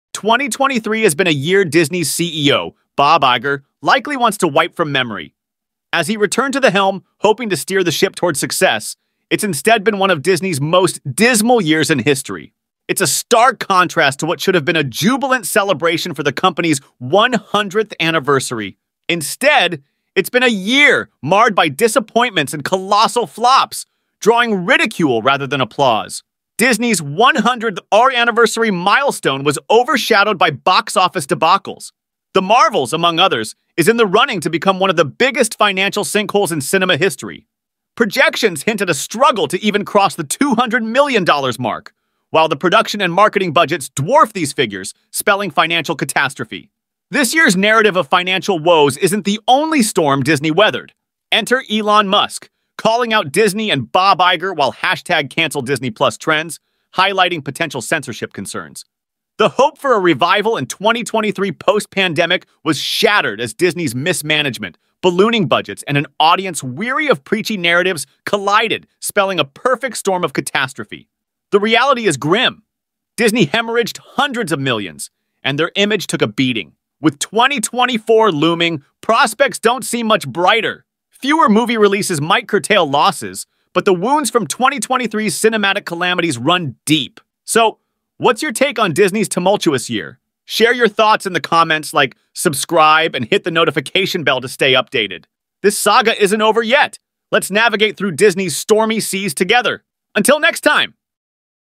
Disney Gets Hit By DISASTER After DISASTER This Entire Year | Disney News Updates
Dive deep into the whirlwind that was Disney's 2023—a year marked not by celebration but by colossal disappointments and financial missteps. Join the conversation as we unpack how Disney's 100th-anniversary year turned into one of the worst in its history, plagued by box office flops and financial woes.

Explore the staggering losses incurred by Disney's big-budget films: The Marvels, Dial of Destiny, Ant-Man of the Was Quantum Mania, and more. These projects, meant to be hits, fell short, leaving the company hemorrhaging millions and the box office figures in tatters.

But that's not all—Elon Musk's challenge to Disney and Bob Iger added another layer of complexity, sparking CancelDisneyPlus trends and raising concerns about potential censorship.

Join us as we break down the harsh reality Disney faced in 2023, the challenges they encountered, and the financial setbacks they endured. The year may have passed, but the repercussions continue. Don't miss this deep dive into Disney's rollercoaster ride of a year!